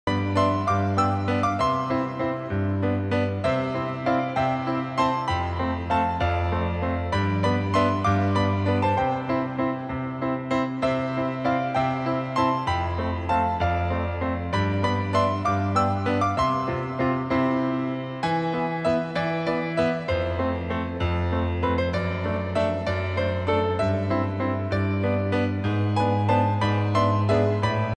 Learn how to draw a lilac flower with one line
Single continuous line drawing a lilac